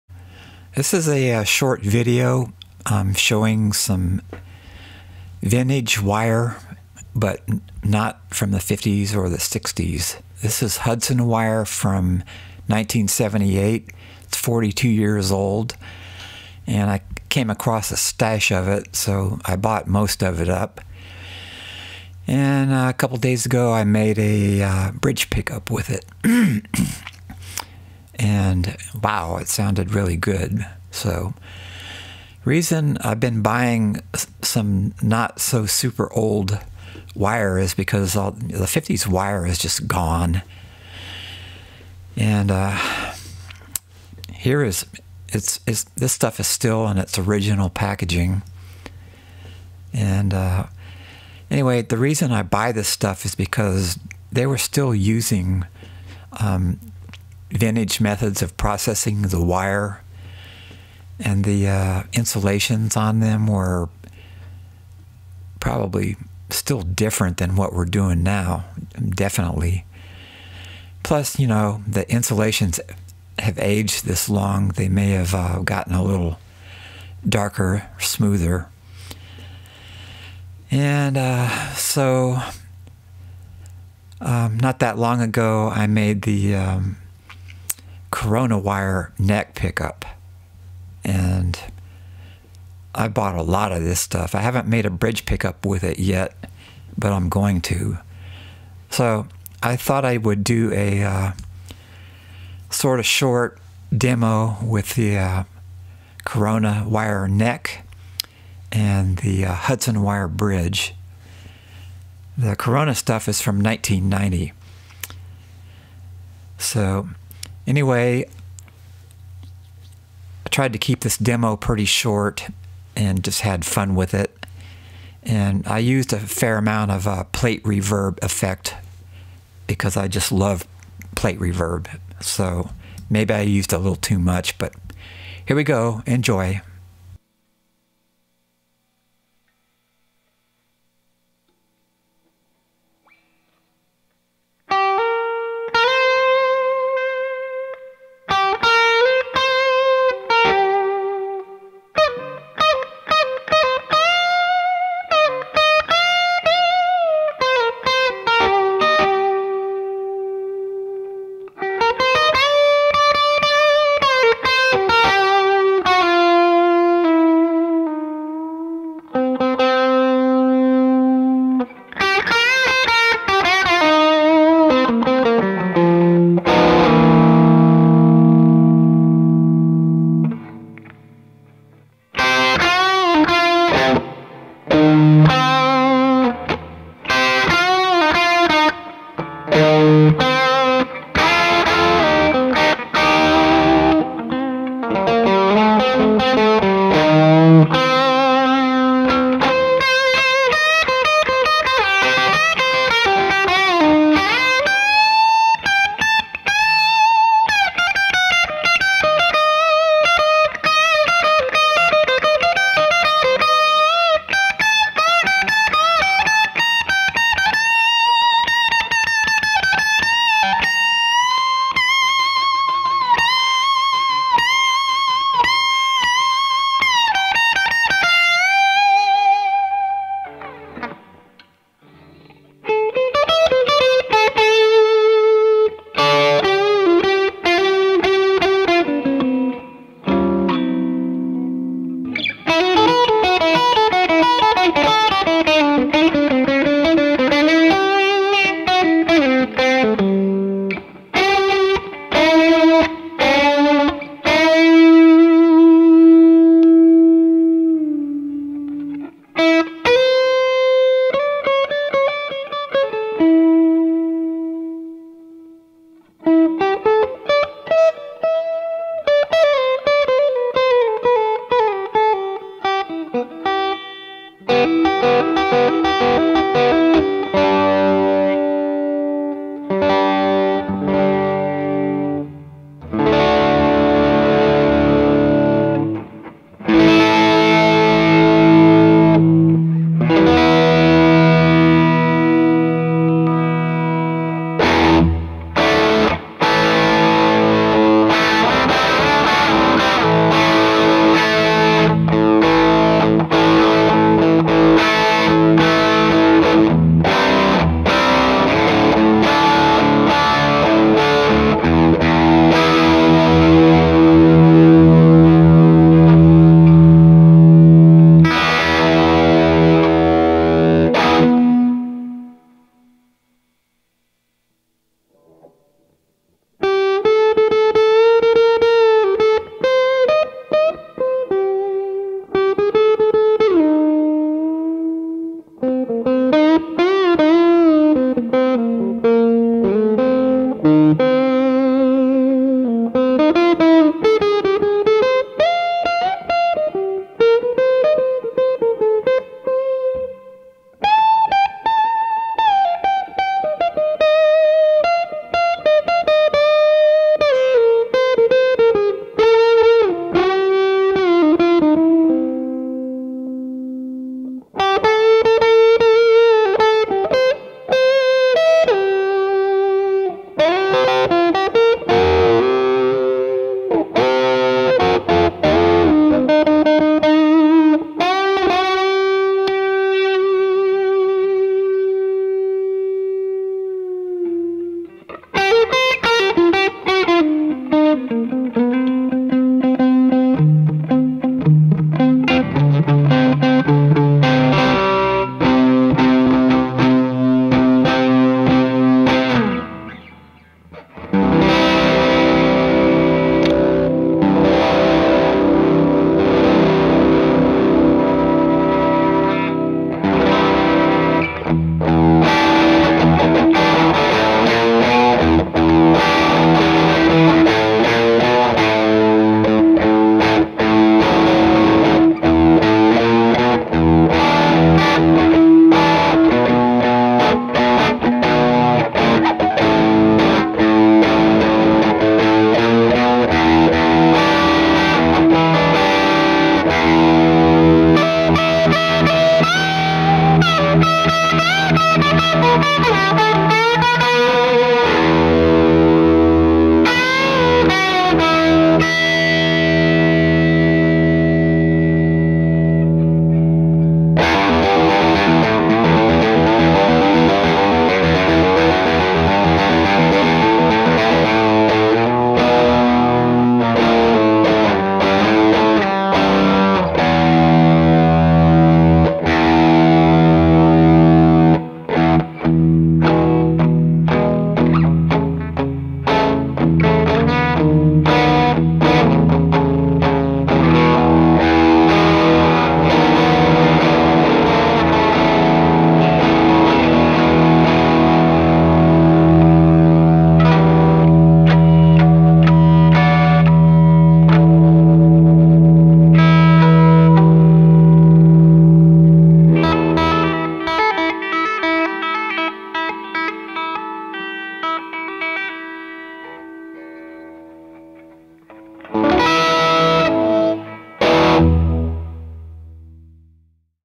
Two NOS Vintage Wire Prototype PAF Replicas
If you saw the 30 year old Corona NY neck PAF replica, well I also have a small stash of 42 year old wire, and finally got around to making a bridge pickup to go with the Corona Neck. This wire is from 1978, 42 years old. I used an A4 magnet in this prototype and liking it alot. Without being able to find anymore 50's wire, I've kept my eyes out for wire going back at least 20 years ago, because we were still making wire the old ways. Our modern wire has now become the least vintage sounding ever. These old rolls just sound fantastic to me, and have enough to be able to experiment with the design, and to be able to offer them for sale until it runs out. I used some plate reverb effect in the mixer, maybe a little too much but I like it more than reverb and have always been using it in demos but much less. Enjoy!
STEPHENS DESIGN PICKUPS ON FACEBOOK, Join me there and please SUBSCRIBE1.
Thanks,
Dave Stephens